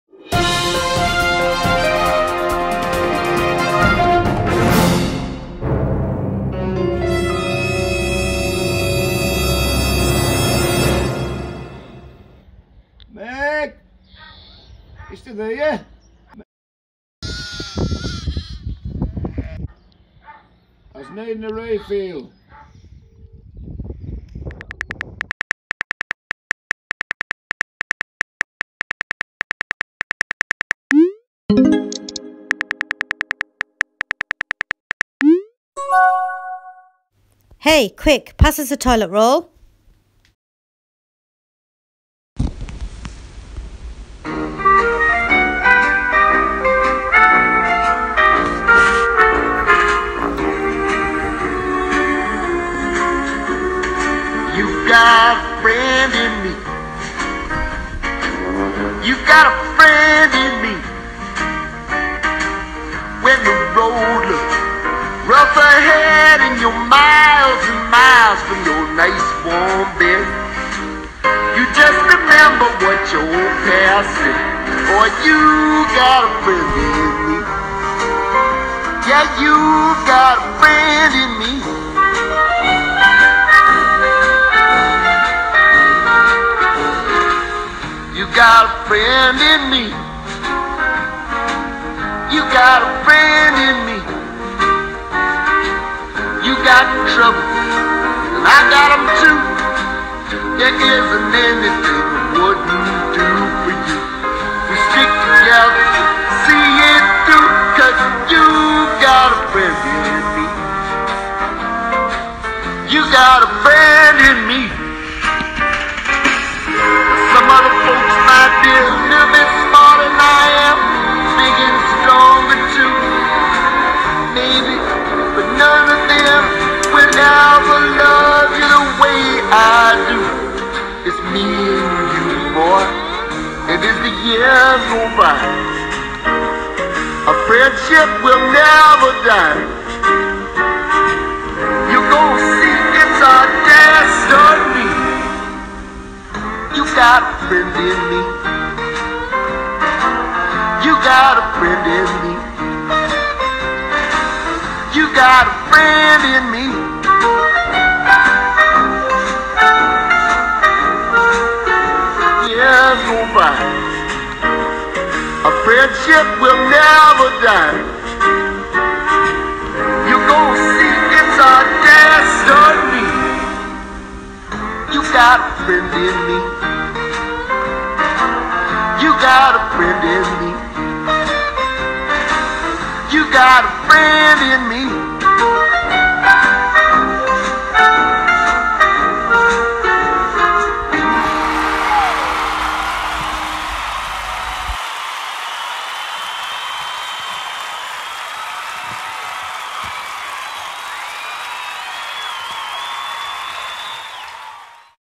A Friend In Need - Coronavirus Lockdown
Fantastic Show of Community Spirit - Our village, Crosby Garrett accepted the toilet roll challenge - Neighbours, families, parents, Grandparents, Children, Dogs, Horses and sheep all showing Amazing Virtual Community Spirit, Having A Laugh ,Staying safe, staying connected and  absolutely rocking the lockdown - Covid -19 Lockdown 2020 won't bring us down!